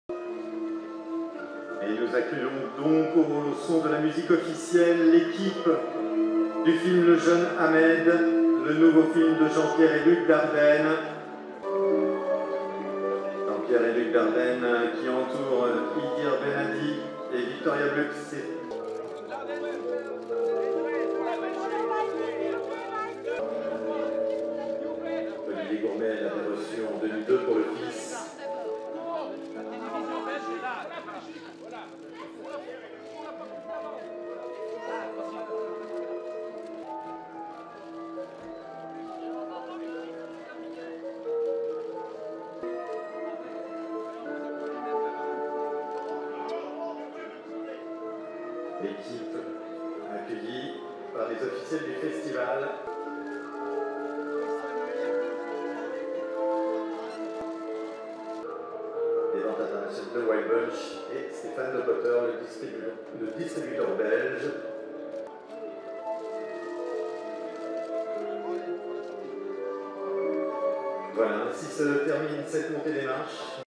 Red carpet time for Dardennes and 'Young Ahmed' cast in Cannes
The cast from "Young Ahmed", directed by Belgian filmmaking-brothers Jean-Pierre and Luc Dardenne, enjoy their red carpet moment in Cannes. "Young Ahmed" is competing for the Palme d'Or at the 2019 Film Festival. IMAGES